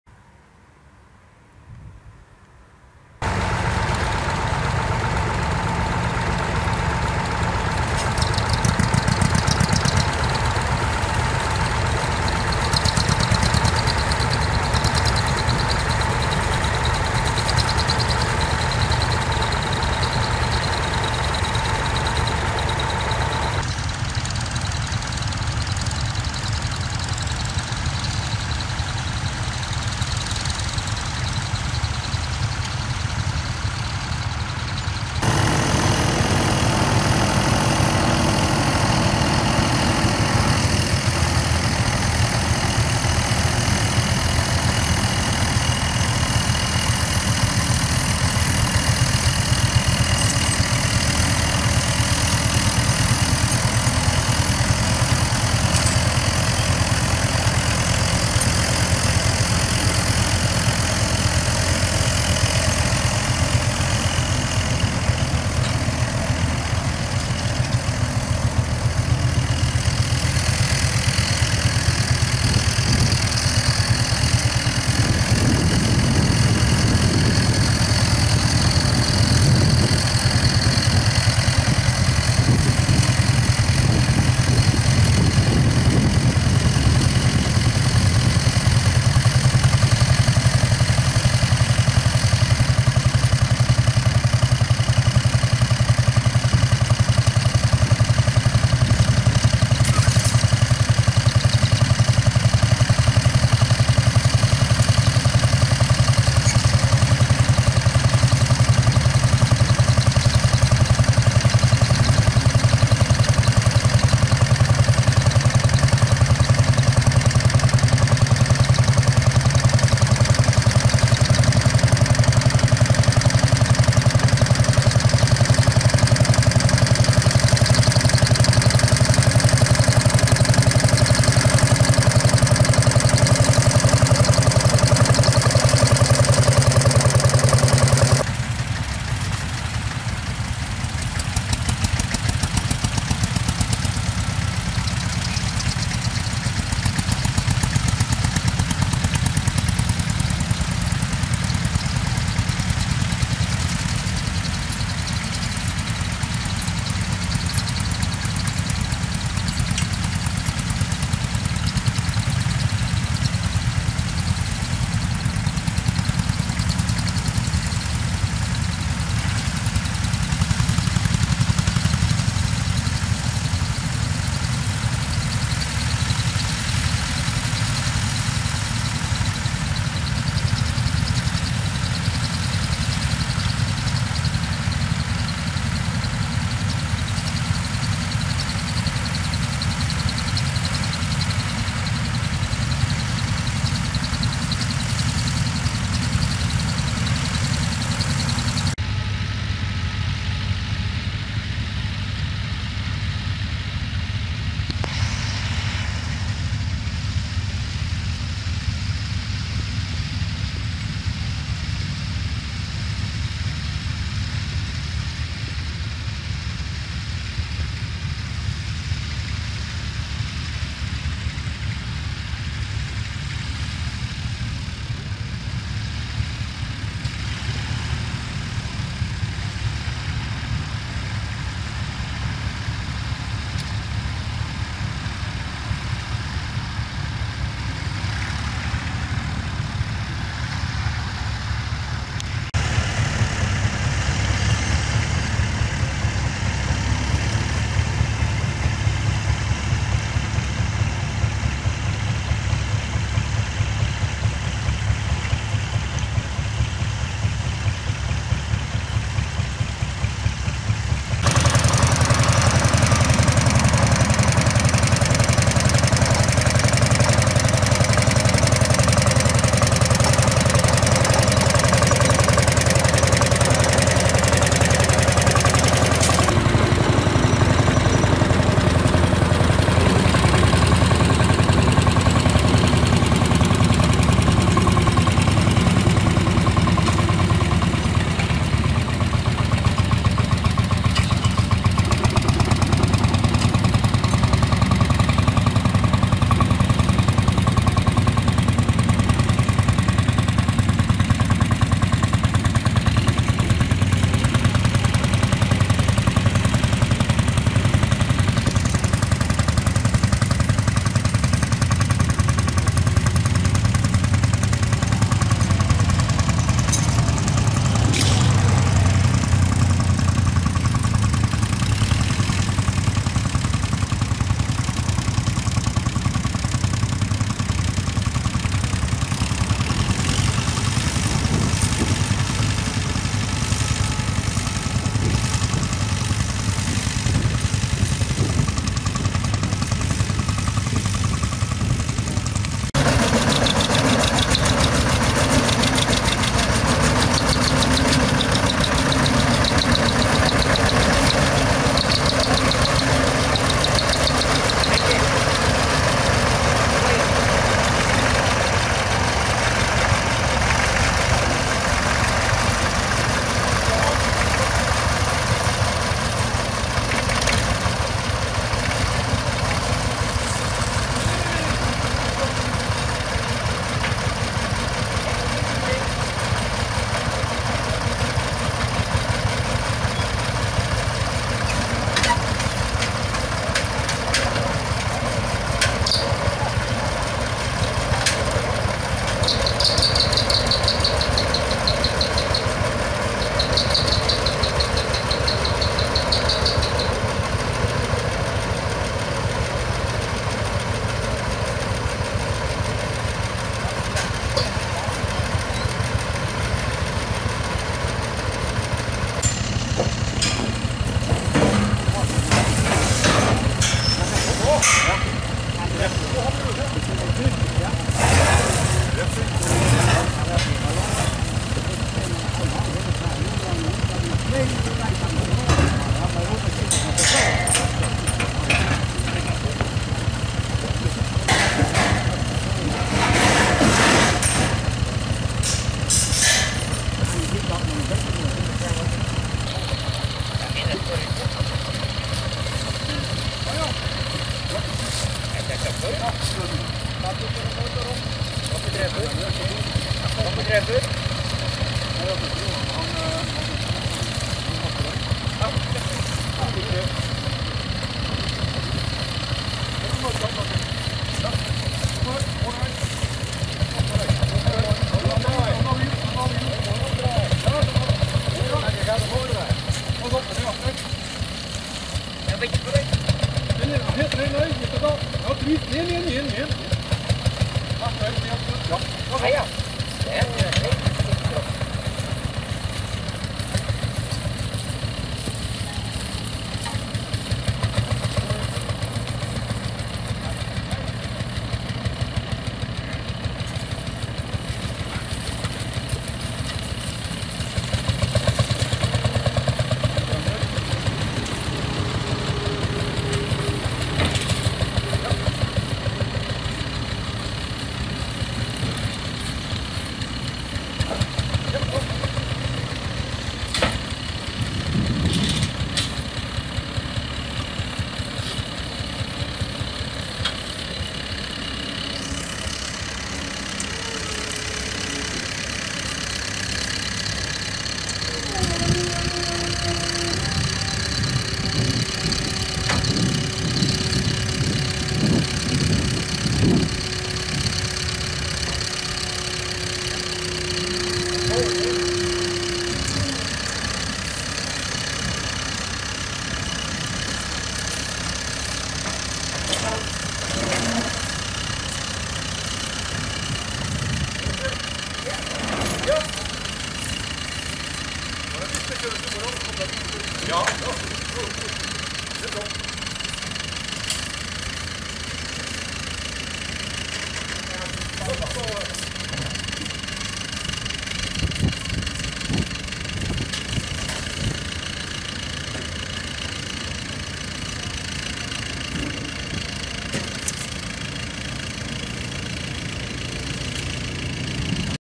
Belgisch kampioenschap 2012
een zeer warm Belgisch kampioenschap oldtimerploegen in Sint-Truiden 2012